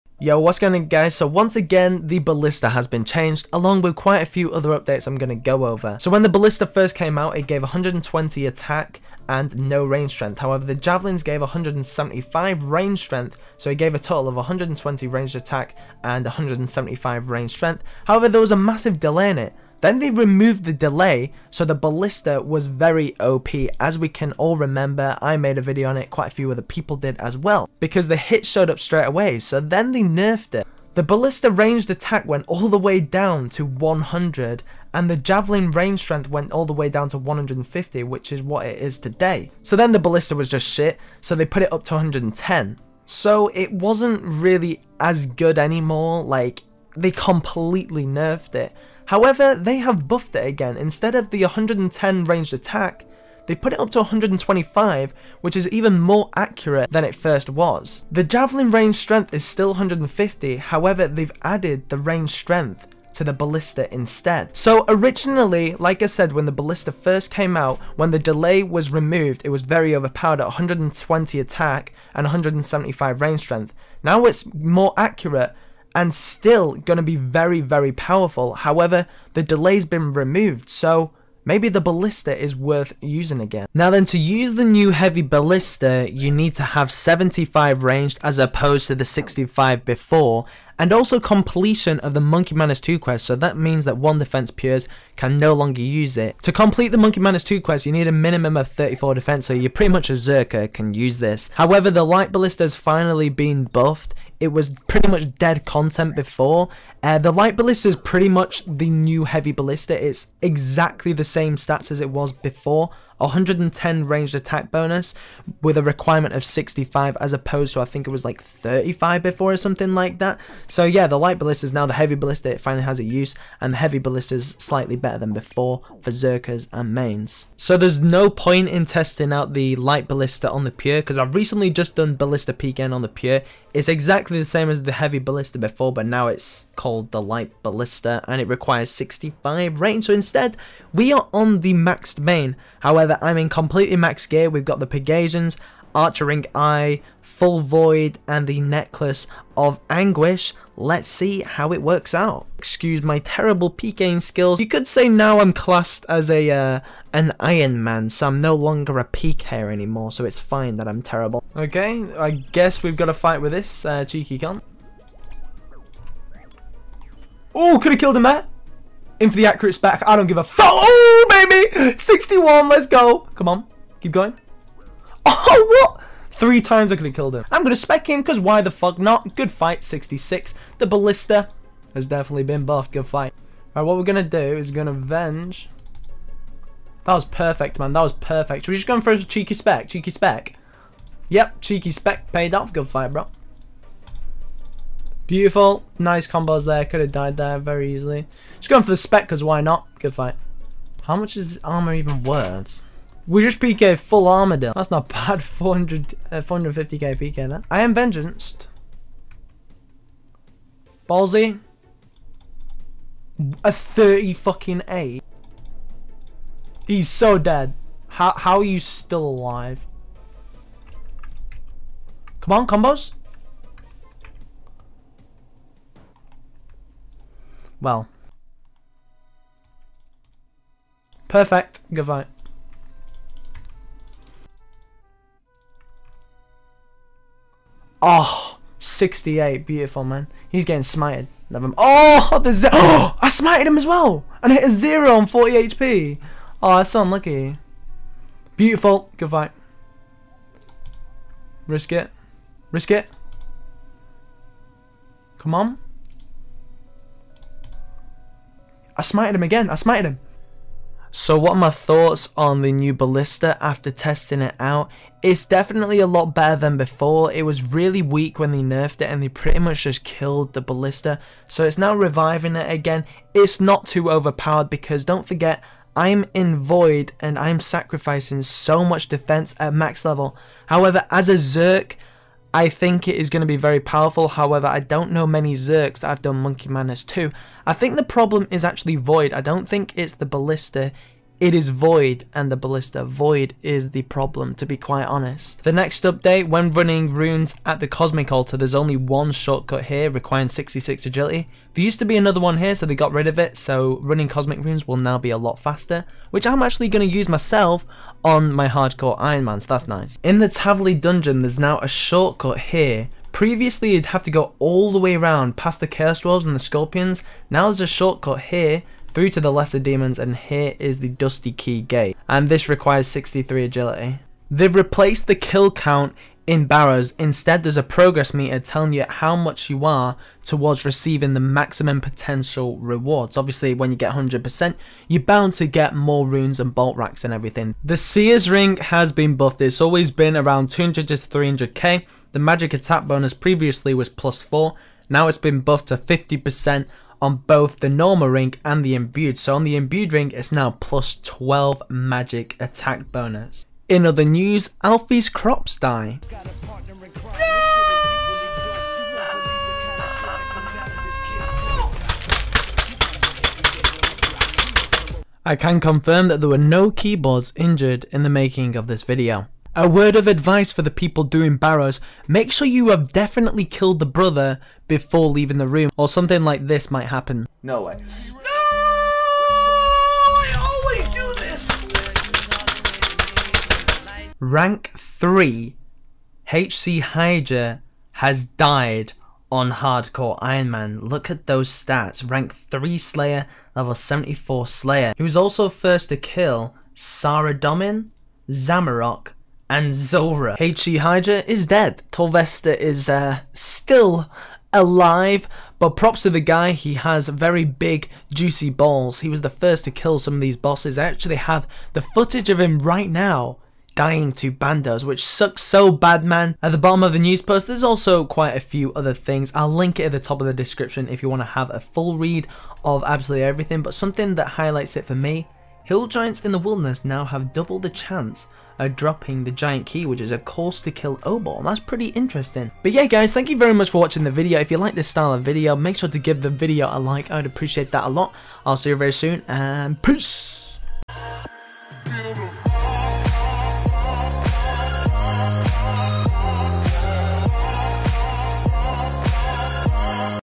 Ballista Buffed AGAIN? (Test)
Ballista once again been buffed, worth using again?